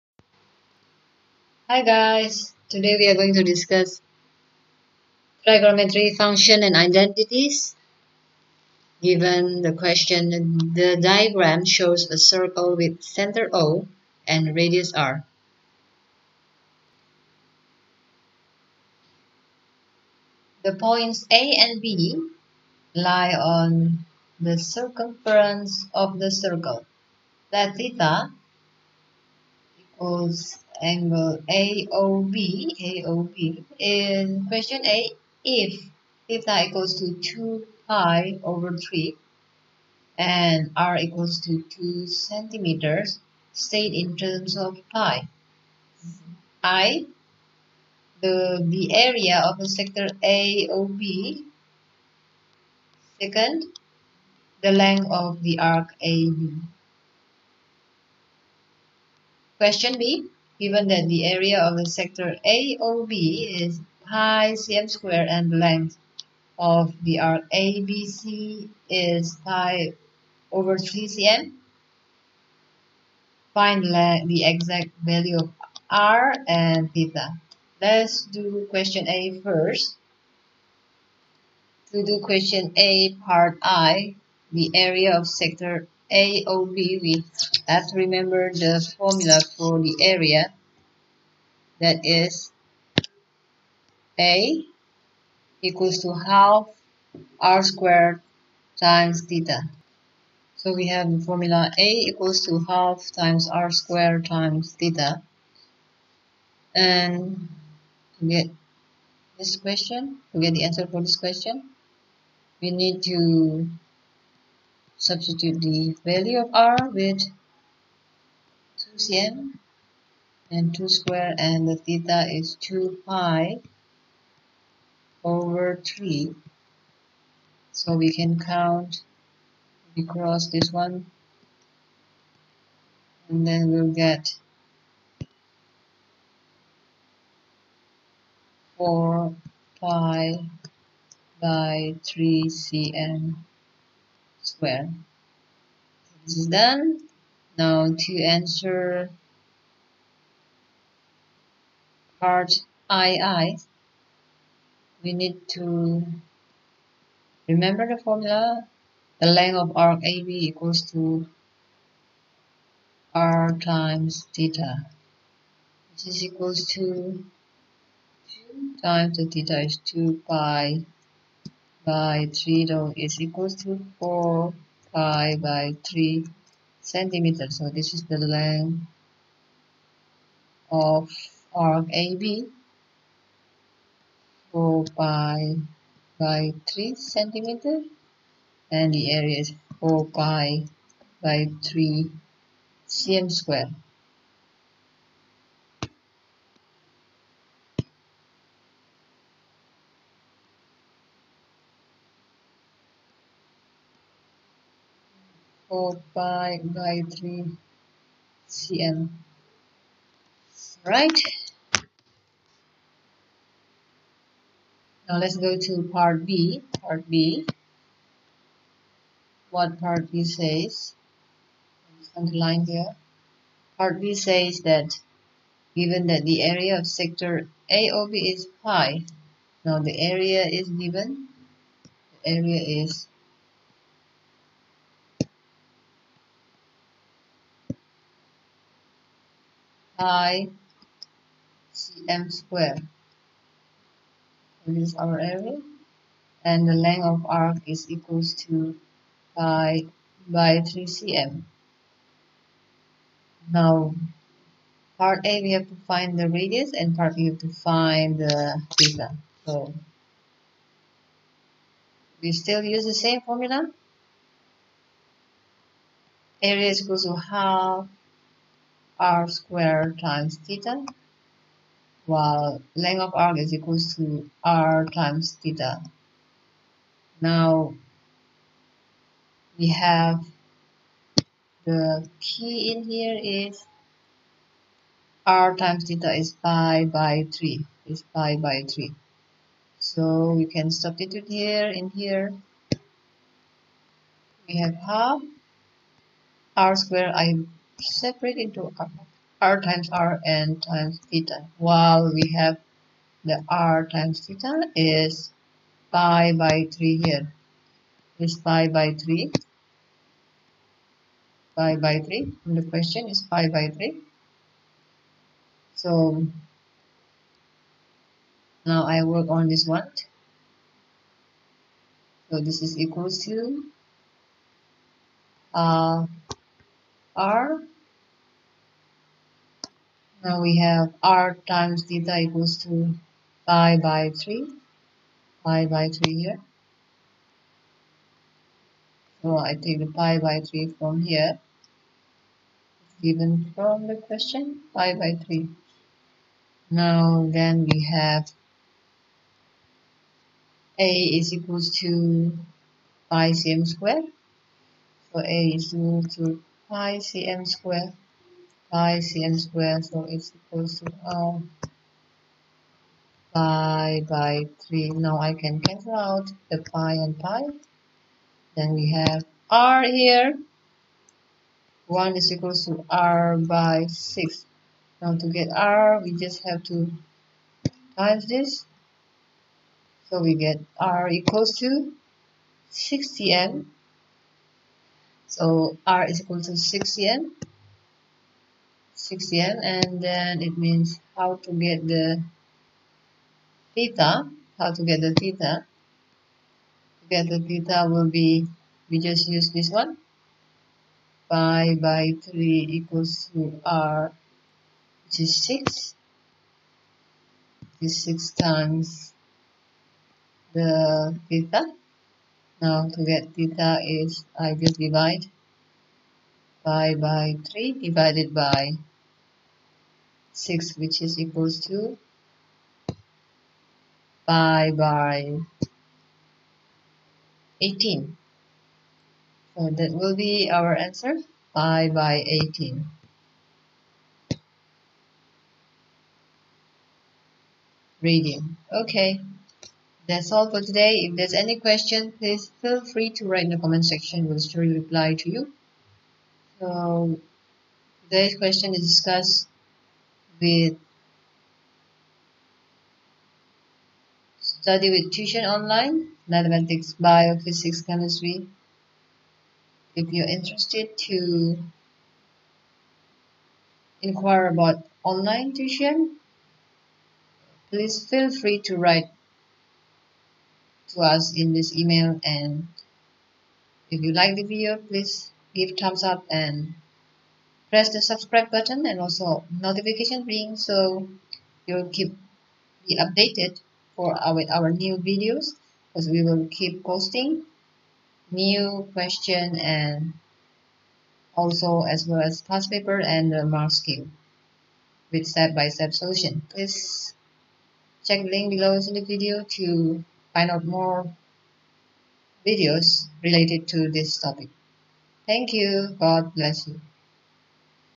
IB Math-SL-the area of sector AOB trigONOMETRY equations-Gr 11&12-solved question-easy to understand
IB_MATHS-SL